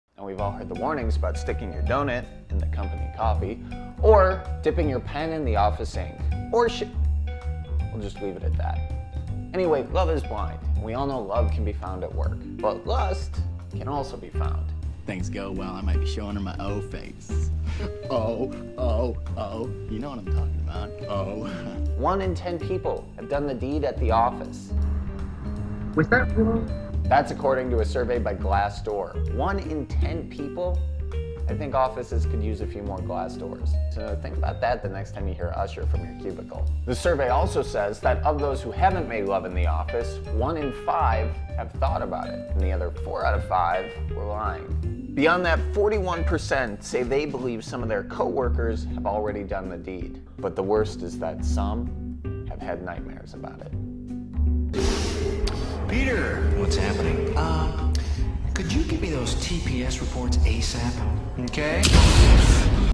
Office Sex: 1 in 10 Admit to Doing the Deed
Paging George Costanza: Twelve percent of people surveyed admit to actually "making love" in the workplace, according to Glassdoor, a jobs and career community. About one in five (22%) say that while they haven't rogered the cashmere-loving cleaning lady or any other co-worker in the office, they have thought about doing it, which seems low. And 41 percent say they believe some of their co-workers have made love in the workplace.